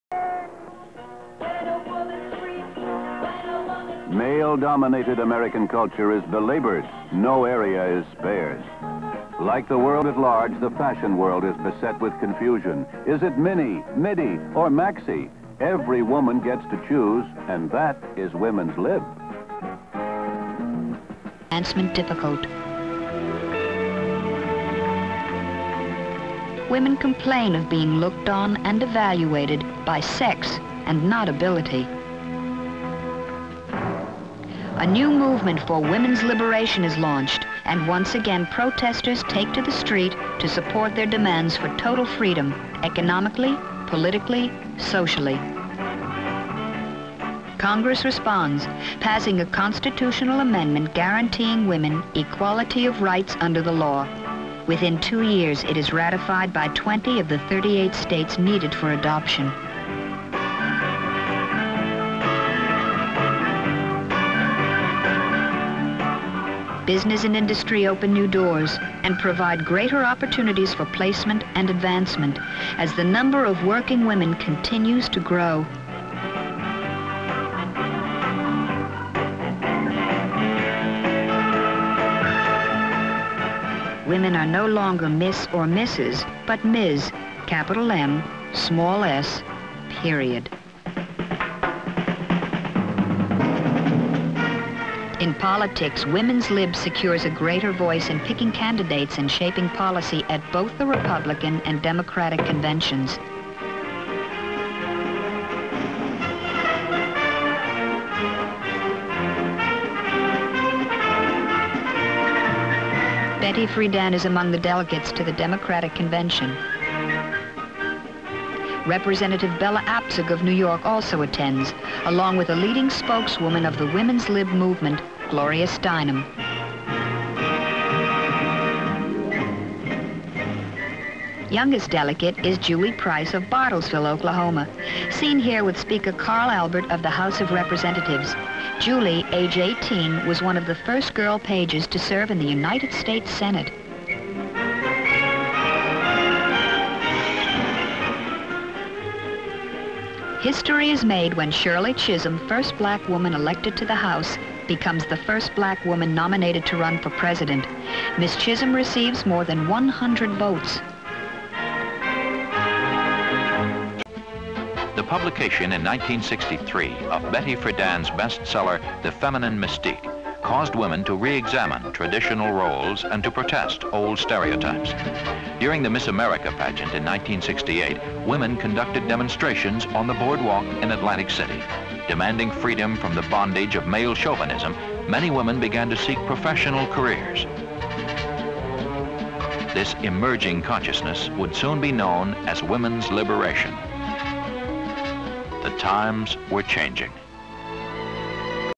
Women's Liberation (stock footage / archival footage)
Statue of Liberty, Women's Lib rallies and protests, Pro-choice demonstration, lots of protest signs. Women's rights marchers; Shots of skirt lengths. Models in photo shoot; Radical Women's libbers; Feminists marching in New York; Women at Democratic and Republican Conventions; CU Betty Friedan, Bella Abzug, Gloria Steinem, Carl Albert, Shirley Chisholm. Sign I AM A WOMAN NOT A TOY PET OR MASCOT Women picket (bra burning); throw away lingerie. Early women's lib march, Atlantic City boardwalk during the 1968 Miss American Pageant: women shout, throw bras in trash can.